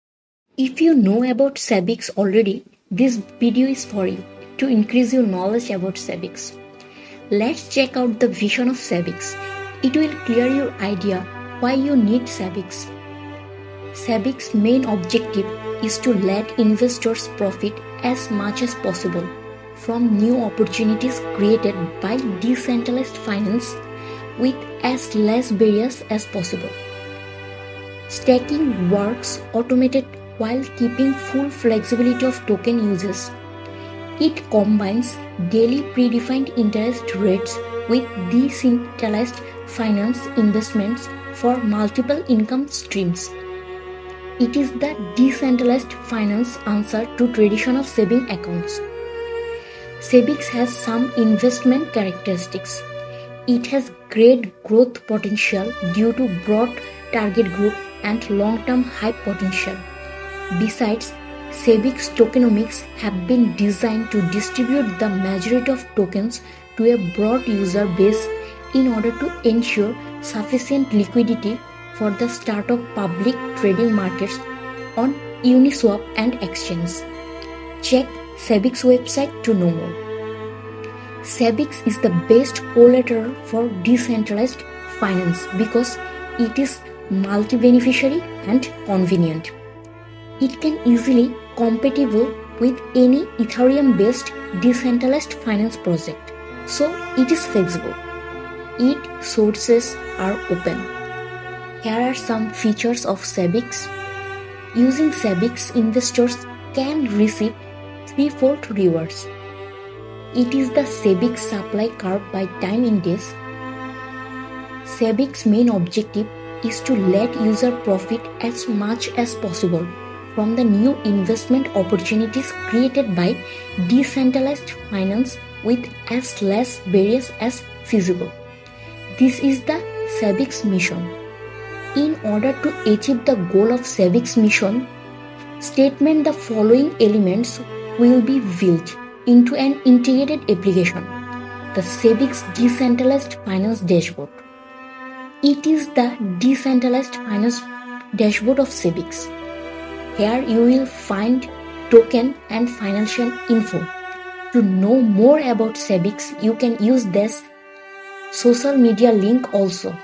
✌✌✌✌Savix✌✌✌✌ and Its Integration with Blockchain Technology
SAVIX (SVX Coin) could be a digital currency Assets that may storm the planet inside the year 2021, dispatched upheld Ethereum Network DeFi that is filling in quality step by step, with the expect to be used by business people, minuscule organizations and new companies for utility capacities.
⚡Official links for more details,
⚡Authorship
BitcoinTalk username : onemancrypto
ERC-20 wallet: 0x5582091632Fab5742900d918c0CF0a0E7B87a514